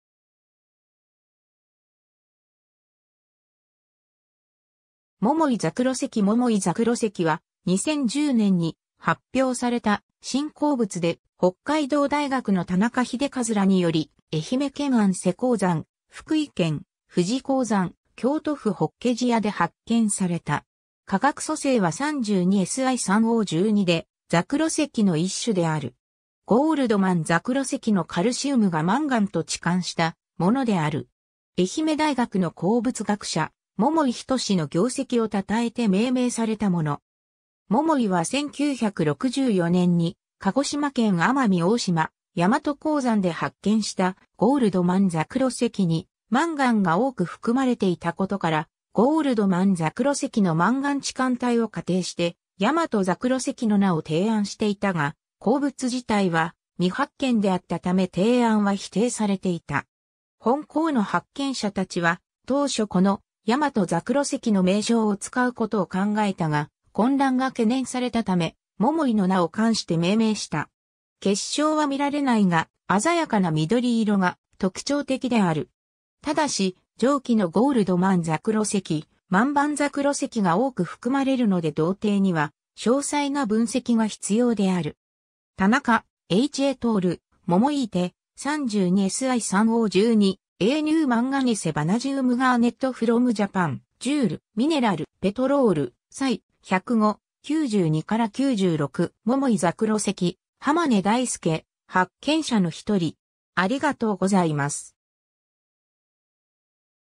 桃井柘榴石
桃井柘榴石 桃井柘榴石（ももいざくろいし、 Momoiite）は、2010年に発表された新鉱物で、北海道大学の田中秀和らにより、愛媛県鞍瀬鉱山・福井県藤井鉱山・京都府法華寺野で発見された。
化学組成は (Mn2+, Ca)3(V3+, Al)2Si3O12 で、柘榴石の一種である。
ゴールドマン柘榴石（灰バナジン柘榴石、Goldmanite）のカルシウムがマンガンと置換したものである。
 愛媛大学の鉱物学者、桃井斉の業績を称えて命名されたもの。
桃井は1964年に鹿児島県奄美大島・大和鉱山で発見したゴールドマン柘榴石にマンガンが多く含まれていたことから、ゴールドマン柘榴石のマンガン置換体を仮定して「大和柘榴石（Yamatoite）」の名を提案していたが、鉱物自体は未発見であったため提案は否定されていた。
本鉱の発見者たちは、当初この「大和柘榴石」の名称を使うことを考えたが、混乱が懸念されたため桃井の名を冠して命名した。
 結晶は見られないが、鮮やかな緑色が特徴的である。
ただし、上記のゴールドマン柘榴石、満礬柘榴石が多く含まれるので同定には詳細な分析が必要である。
 ^ Tanaka, H. et al. (2010): Momoiite, (Mn2+, Ca)3(V3+, Al)2Si3O12, a new manganese vanadium garnet from Japan. Jour. Mineral. Petrol. Sci., 105, 92-96. ^ 桃井ざくろ石、浜根大輔（東京大学物性研究所電子顕微鏡室）、発見者の一人